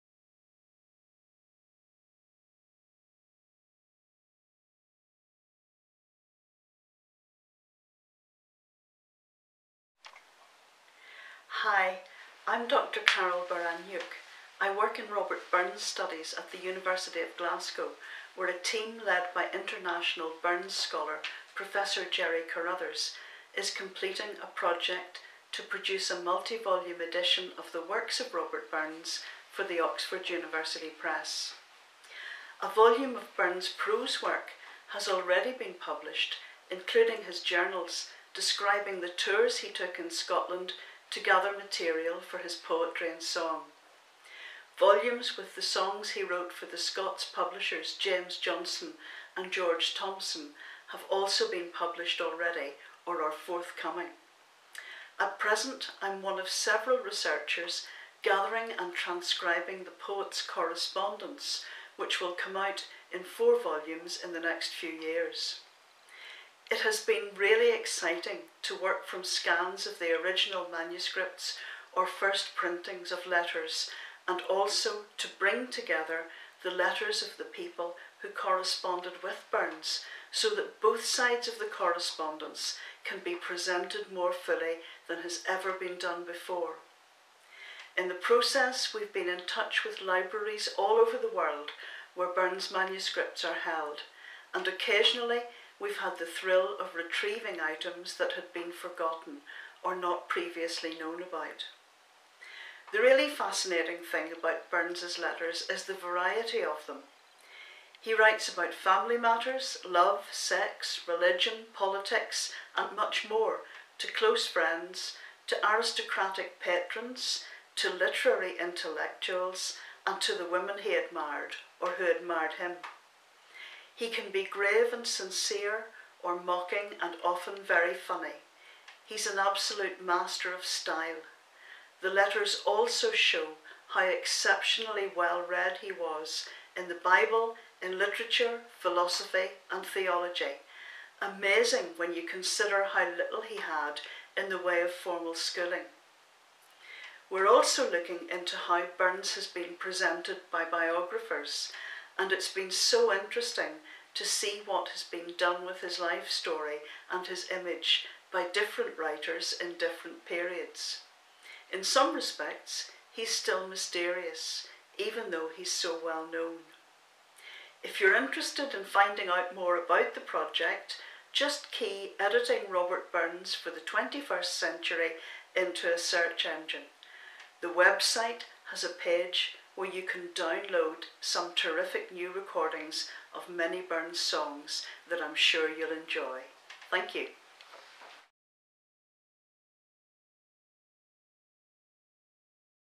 The Centre for Robert Burns Studies with Dr Carol Baraniuk
Join Dr Carol Baraniuk from the Centre for Robert Burns Studies at the University of Glasgow as she shares some exciting developments regarding the Centre and a project called, 'Editing Robert Burns for the 21st Century'.

Dr Carol Baraniuk is Research Associate in Robert Burns Studies at the University of Glasgow, working on the project ‘Editing Robert Burns for the 21st Century’ which is funded by the AHRC. She has published and presented widely on Scottish poetic traditions, with a particular focus on Burns’s influence on eighteenth and nineteenth-century poets in the north of Ireland who wrote in Scots.

Currently, she is researching Burns’s correspondence for forthcoming volumes of the Oxford University Press edition, locating and transcribing many of the manuscripts of his letters and those of individuals who wrote to him. Carol is preparing a close study of Burns’s employment of Biblical quotations in his letters. She also has in preparation an essay on the poet’s biographers in the twentieth and twenty-first centuries for the forthcoming Oxford Handbook to Robert Burns (edited by Gerard Carruthers).